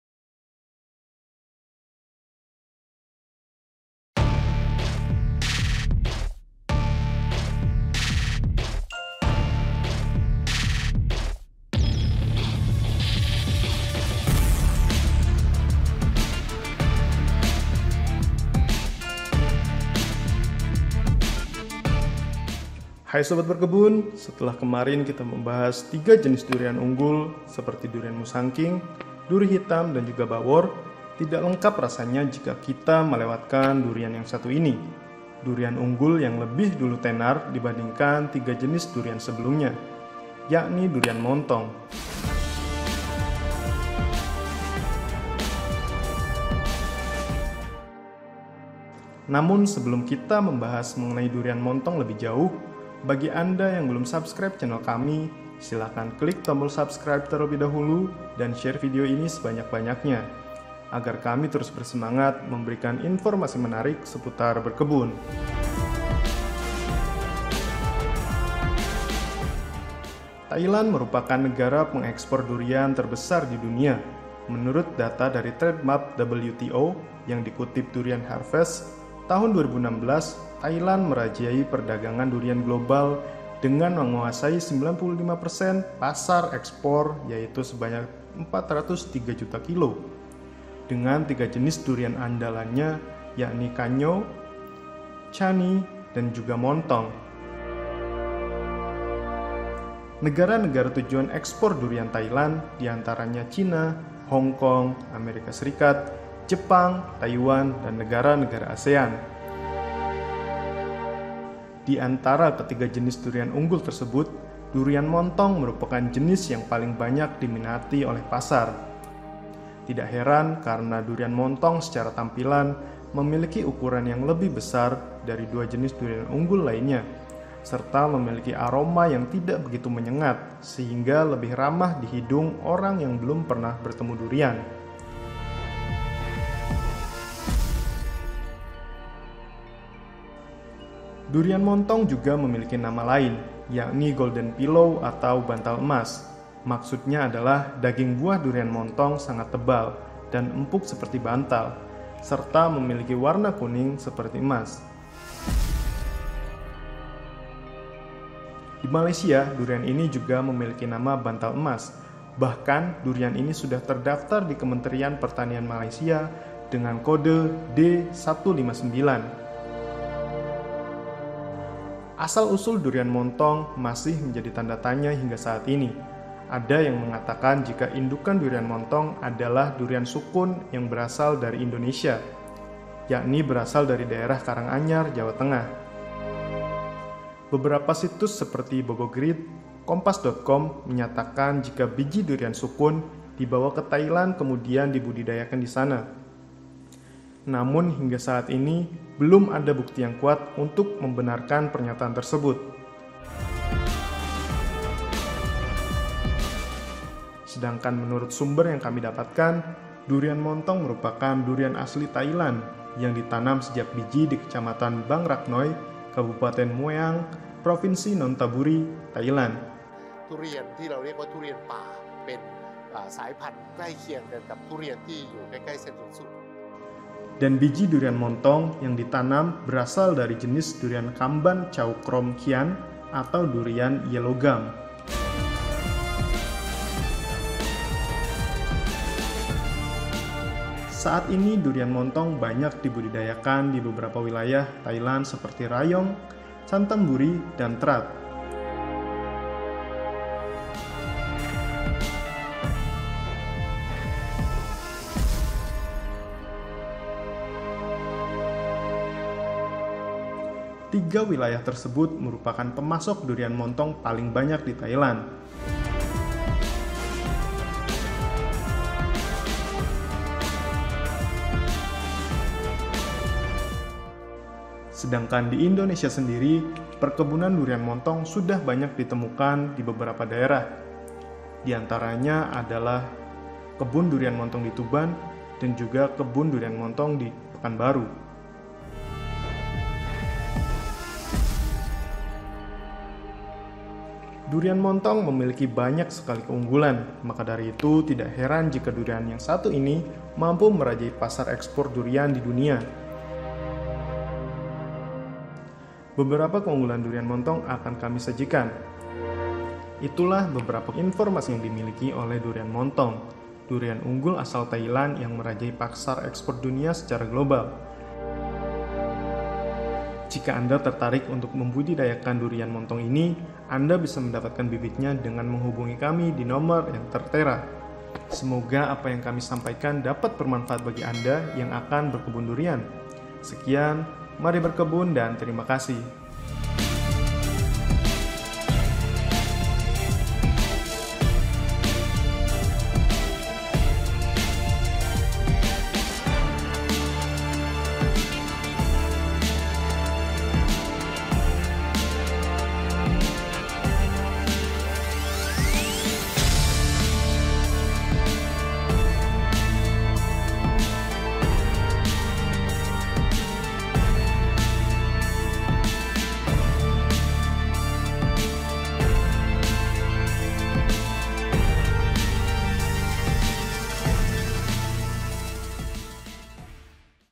Ulasan Lengkap tentang Ekspor Durian Monthong ke Negara Asia Tenggara dan Amerika
Jika anda tertarik untuk menanam atau mengebunkan DURIAN ini anda bisa mendapatkan bibitnya dengan menghubungi kami lewat nomer:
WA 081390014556

Thailand merupakan negara pengekspor durian terbesar di dunia, Menurut data dari Trade Map WTO yang dikutip DurianHarvests, tahun 2016 Thailand merajai perdagangan durian global dengan menguasai 95% pasar ekspor yaitu sebanyak 403 juta kilo. Dengan 3 jenis durian andalannya yakni kanyao, chanee, dan juga montong. Negara2 tujuan ekspor durian Thailand diantaranya China,  Hongkong, Amerika serikat, Jepang, Taiwan, dan negara-negara ASEAN.
Diantara ketiga jenis durian unggul tersebut, durian montong merupakan jenis yang paling banyak diminati oleh pasar. Tidak heran karena durian montong secara tampilan memiliki ukuran yang lebih besar dari 2 jenis durian unggul lainnya. Serta memiliki aroma yang tidak begitu menyengat sehingga lebih ramah di hidung orang yang belum pernah bertemu durian.
Durian montong juga memiliki nama lain yakni golden pillow atau bantal emas. Maksudnya adalah daging buah durian montong sangat tebal dan empuk seperti bantal serta memiliki warna kuning seperti emas.
Menurut sumber yang kami dapatkan durian montong merupakan durian asli Thailand yang ditanam sejak biji di Kecamatan Bang Rak Noi, Kabupaten Mueang, Provinsi Nonthaburi, Thailand. Dan biji durian montong yang ditanam berasal dari jenis durian kamban Chao Krom Khian atau durian yellow gum.
Saat ini durian montong banyak dibudidayakan di beberapa wilayah Thailand seperti Rayong, Chanthaburi, and Trat. 3 wilayah tersebut merupakan pemasok durian montong paling banyak di Thailand.
Keunggulan Durian Montong
Durian montong memiliki banyak sekali keunggulan, maka dari itu tidak heran jika durian yang satu ini mampu merajai pasar ekspor durian di dunia. Beberapa keunggulan durian montong adalah sebagai berikut.
1. Memiliki Ukuran buah yang besar
Durian montong memiliki ukuran buah yang besar jika dibandingkan dengan jenis durian kebanyakan. Rata-rata ukuran durian montong berkisar antara 4-5 kg per buah, namun mampu mencapai berat maksimal hingga 10 kg satu buahnya.
2. Daging buah tebal dan padat
Daging buah durian montong tebal dengan ukuran biji yang kecil ditambah lagi tekstur buahnya juga padat dan tidak lembek saat dimakan.
3. Rasa yang Dominan Manis
Berbeda dengan jenis durian kebanyakan yang memiliki rasa manis, legit, dan ada sedikit pahit. Durian montong memiliki rasa yang dominan manis tanpa rasa pahit sedikitpun. Rasa durian yang seperti ini jauh lebih disukai oleh kalangan orang-orang barat yang tidak suka dengan rasa pahit pada buah. Karena mereka memiliki stigma jika buah memiliki rasa pahit biasanya mengandung zat karsinogenik yang dapat menyebabkan kanker. Padahal rasa pahit pada durian bukan berasal dari zat tersebut dan tidak ada kaitannya dengan penyakit kanker.
4. Aromanya lebih lembut
Aroma durian montong tidak menyengat seperti durian lokal, aroma durian yang lembut juga lebih mudah diterima oleh orang yang belum pernah bertemu durian sama sekali. Sehingga bagi orang yang tidak menyukai durian karena aroma buahnya yang menyengat, tidak akan merasa terganggu dengan aroma durian montong.
5. Dapat dikirim menggunakan pesawat
Durian montong adalah jenis durian pertama yang boleh naik pesawat. syarat agar buah durian dapat dikirim menggunakan pesawat adalah tidak mengeluarkan bau. Sehingga banyak petani yang memanen durian montong dalam keadaan masih mengkal saat akan di ekspor ke luar negeri. Buah durian montong yang masih mengkal tidak memiliki bau yang menyengat sehingga bisa dikirim menggunakan pesawat. Kemudian durian yang masih mengkal juga memungkinkan durian memiliki daya simpan yang lebih lama. Walaupun saat ini sebenarnya beberapa durian lain sudah bisa dikirim menggunakan pesawat juga dengan cara membungkusnya dengan plastic hingga tidak mengeluarkan bau.
6. Memiliki Nilai Ekonomi Tinggi
Harga buah durian montong lebih mahal jika dibandingkan dengan durian lokal. Untuk harga per kilo durian montong dibanderol dengan harga sekitar Rp. 50.000 – 75.000. untuk satu buah durian montong ukurannya sekitar 3 – 4 kg per buah. Tetapi jika dibandingkan dengan durian import lain seperti musangking, harga durian montong jauh lebih murah. Namun memiliki harga yang lebih tinggi jika dibandingkan dengan durian lokal kebanyakan.